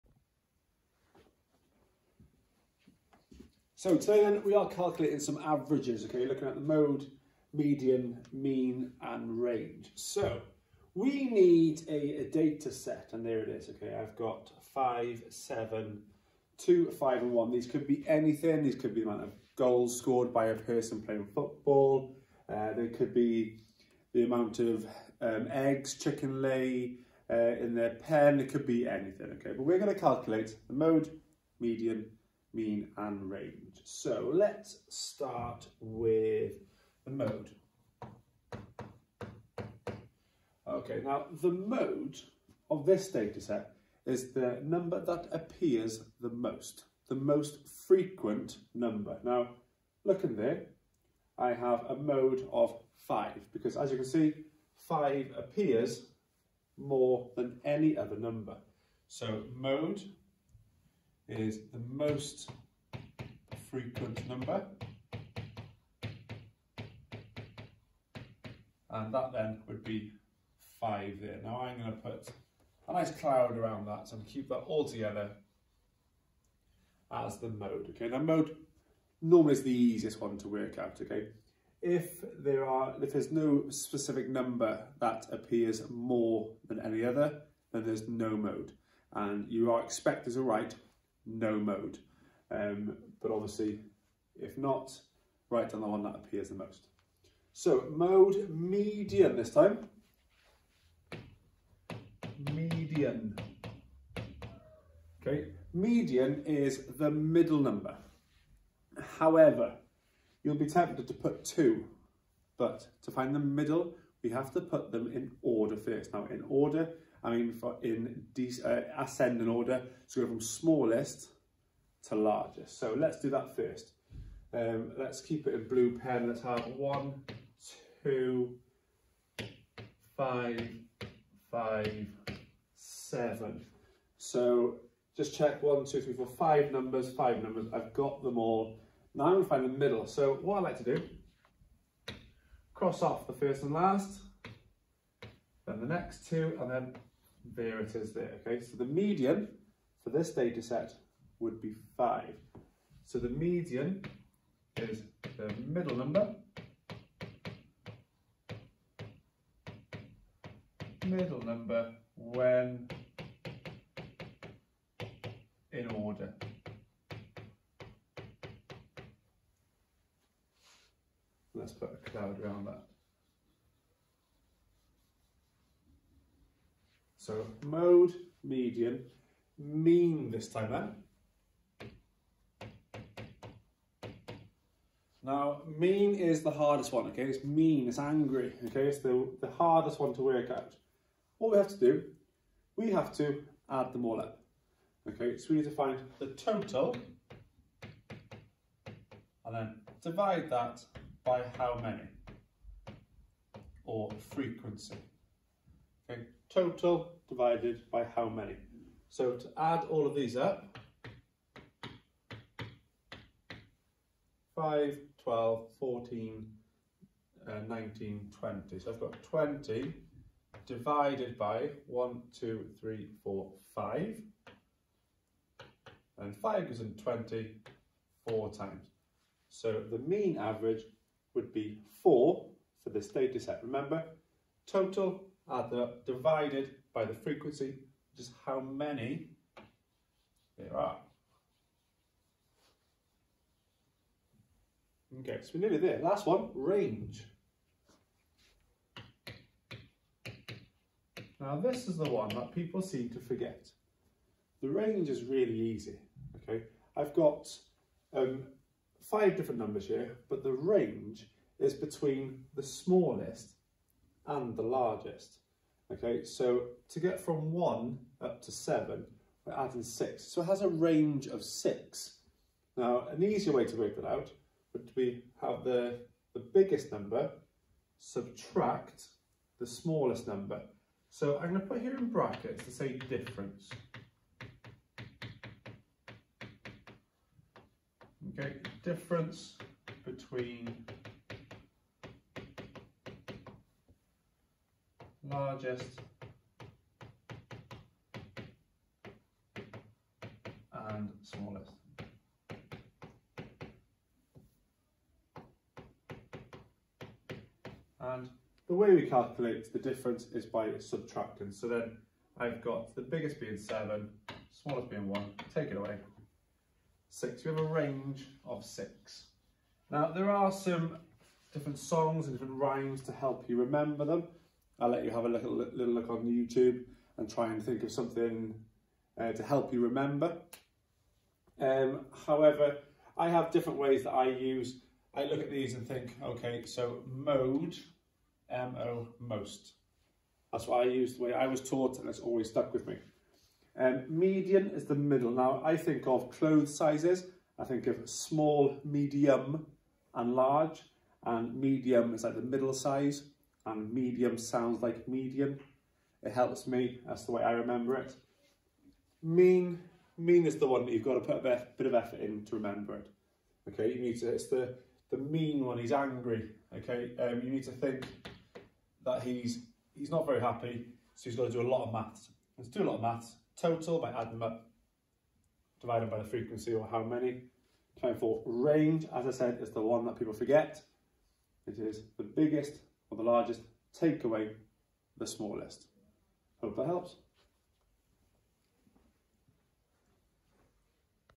GCSE Maths - Mode, Median, Mean and Range
Calculating the mode, median, mean and range from a data set.

If you have two numbers left when calculating the median then you must find the middle of those two numbers.